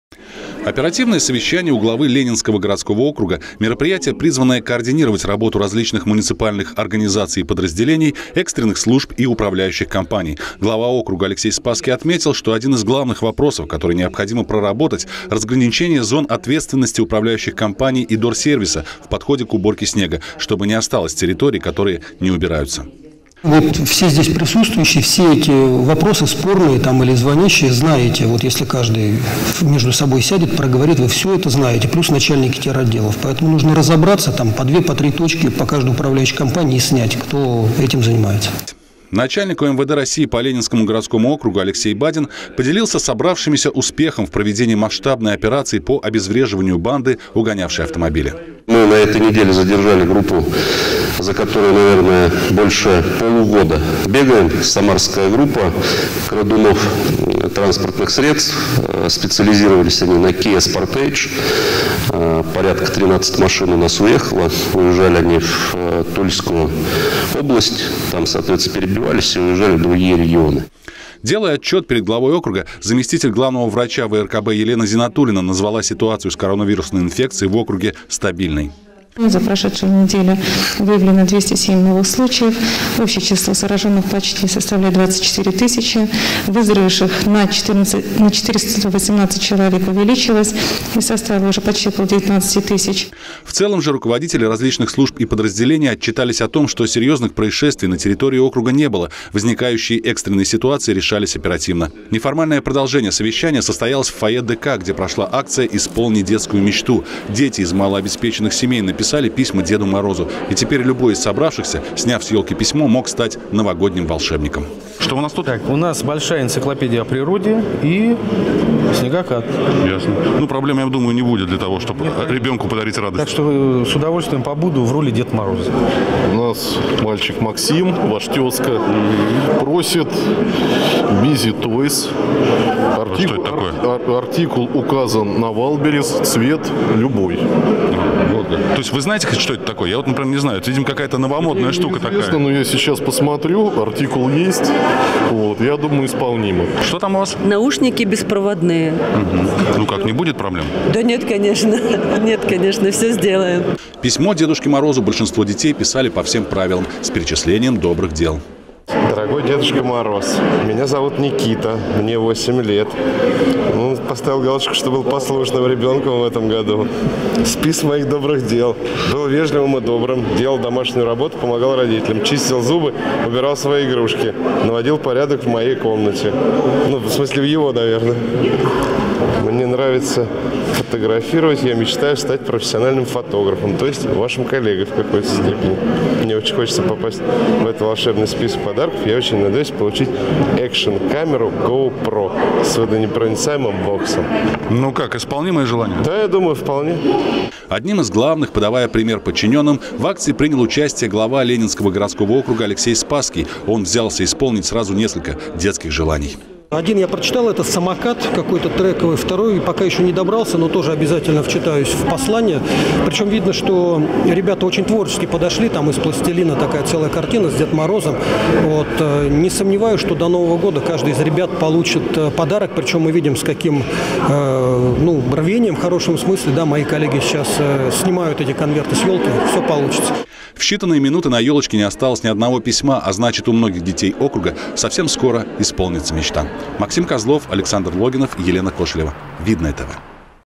ОПЕРАТИВНОЕ СОВЕЩАНИЕ
Очередное оперативное совещание провёл глава Ленинского городского округа Алексей Спасский, в рамках которого прошла и неформальная предновогодняя акция.